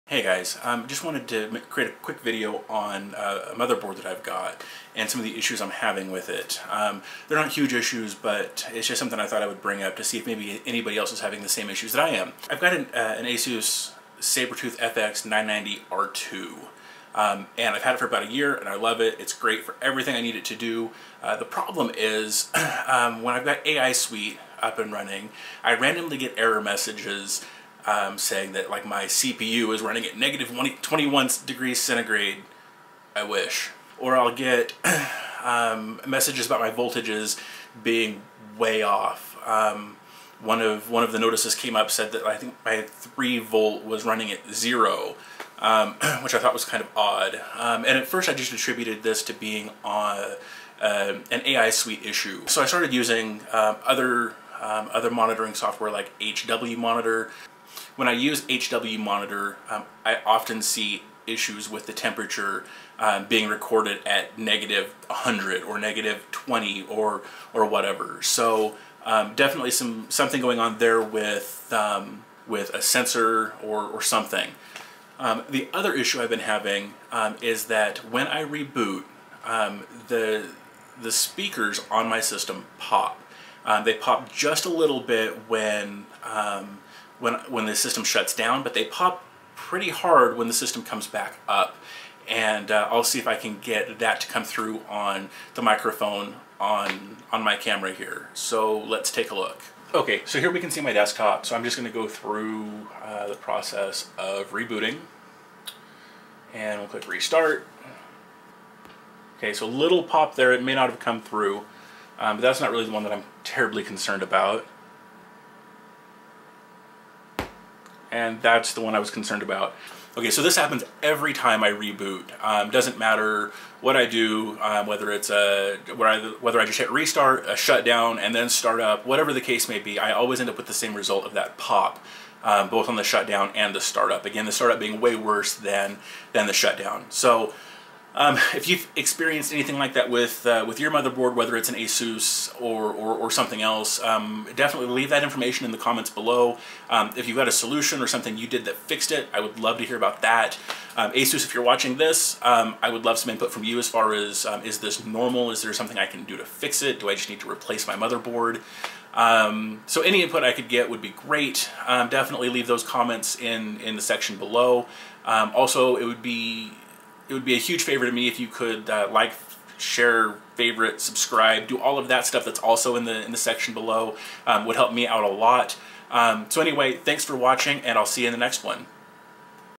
Asus SABERTOOTH 990FX R2.0 Motherboard Sensor & Audio Issues
I've had my Asus SABERTOOTH 990FX R2.0 for just over a year and have had issues with the sensors reporting incorrect numbers as well as popping sounds coming from the speakers when I restart my system. My drivers are all up-to-date.